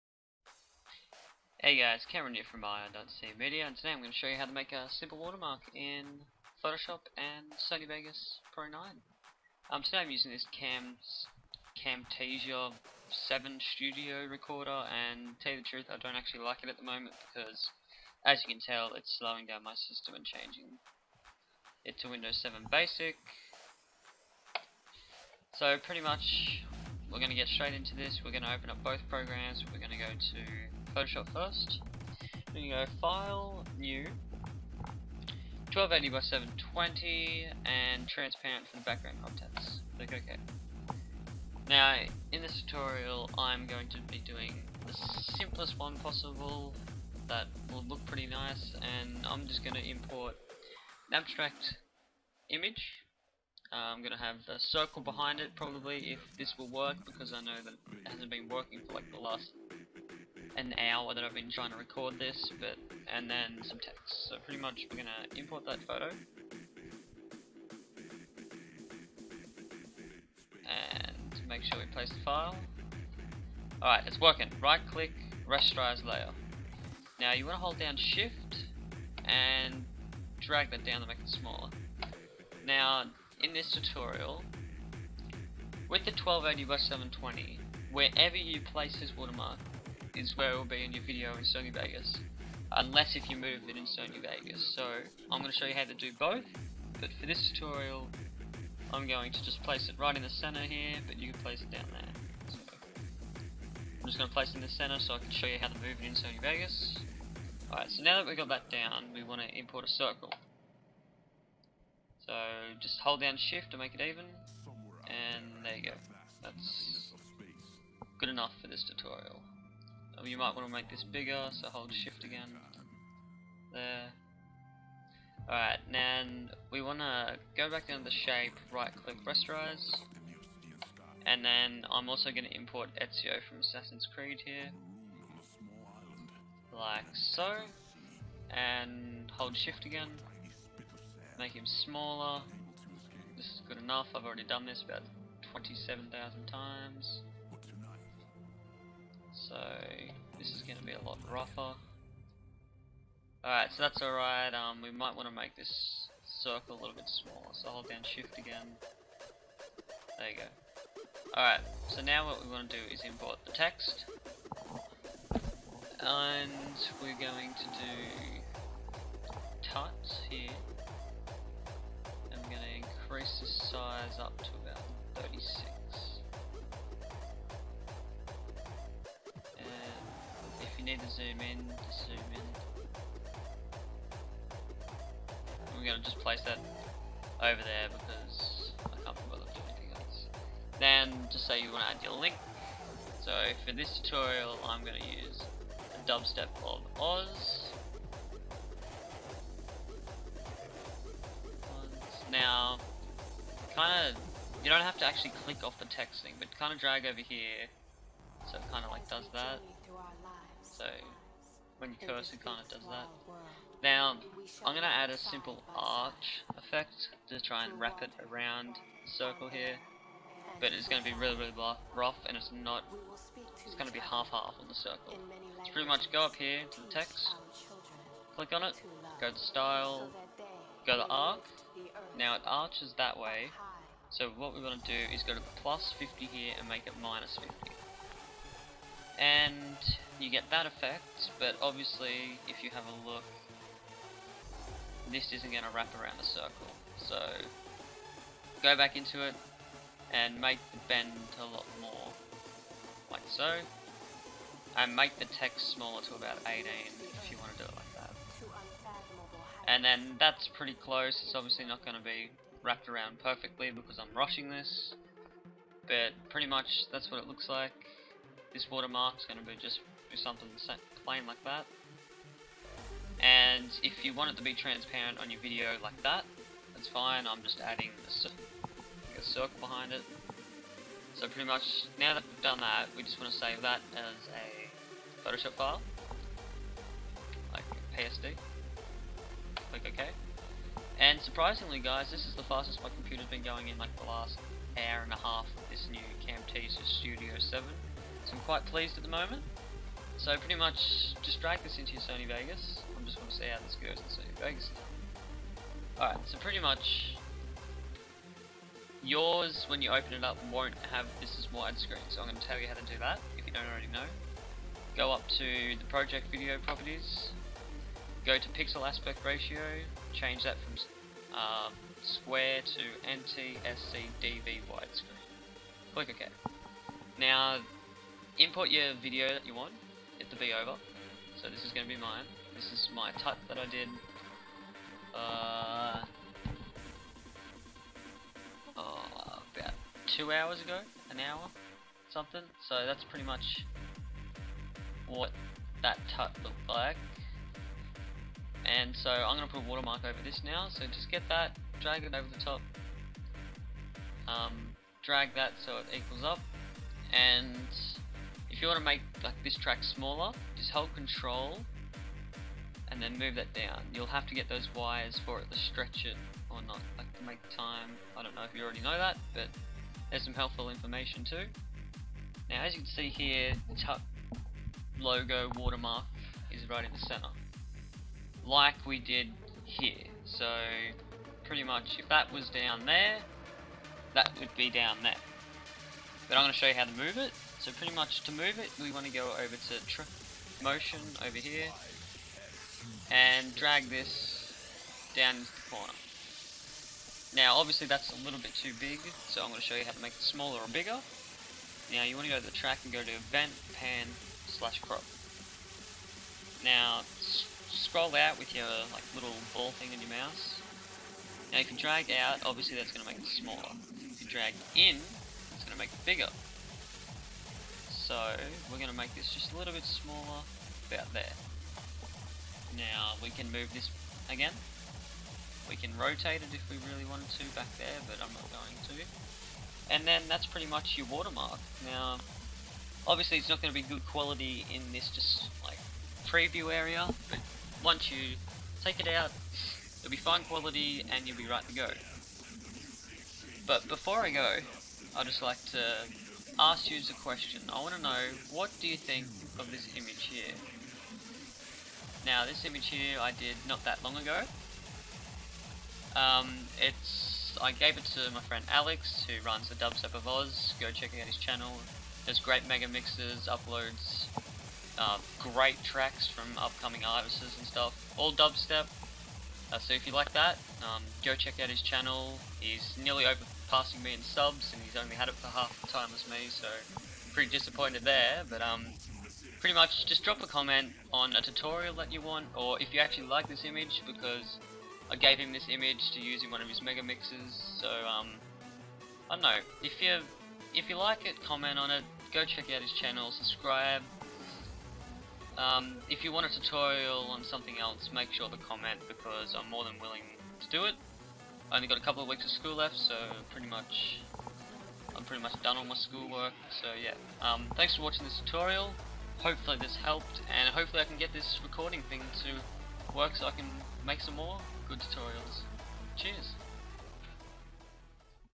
How To Make A Simple Water-Mark Using Photoshop - Sony Sony Vegas
In this tutorial Ill show you how to make a simple watermark using photoshop and sony vegas pro,